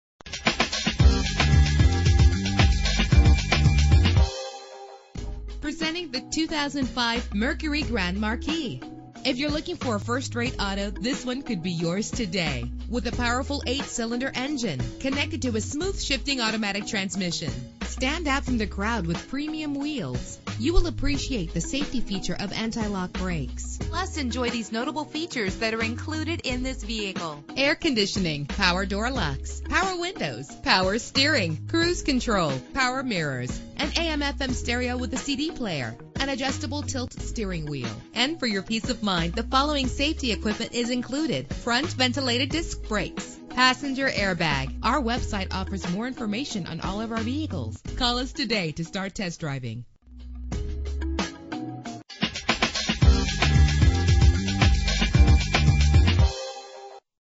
2005 Mercury Grand Marquis - ANDERSON SC
Year: 2005
Make: Mercury
Model: Grand Marquis
Trim: LS
Engine: 4.6 liter 8 cylinder 16 valve
Transmission: 4-Speed Automatic
Color: Silver
Mileage: 71751
Address:
3900 CLEMSON BLVD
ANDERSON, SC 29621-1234